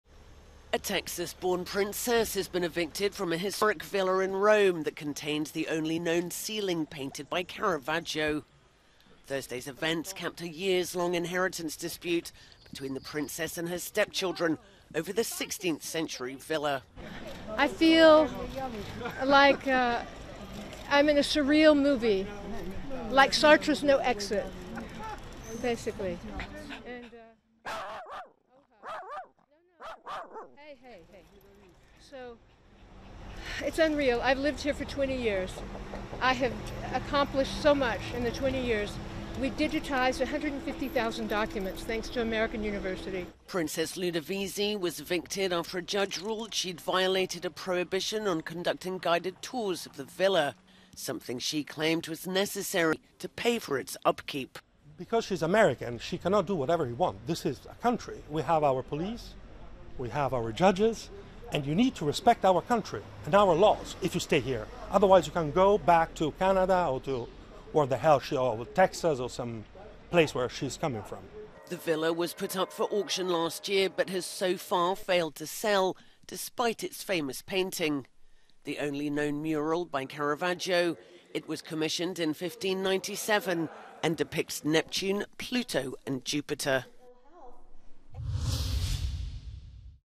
Princess evicted from villa containing only known Caravaggio ceiling
Texas-born princess Rita Jenrette Boncompagni Ludovisi was escorted off the property by police after years of dispute over the property.